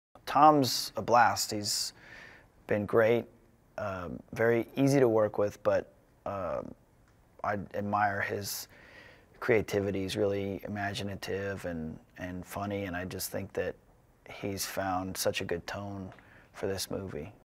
Spider-Man: No Way Home: Why Context is key.
This clip is taken from a Boss Baby interview where Tobey Maguire talks about an entirely different Tom. But by simply changing the Boss Baby Logo to something else, it could mean something totally different.

And yes I know the editing is sloppy but its meant to get a point across not be a blockbuster movie.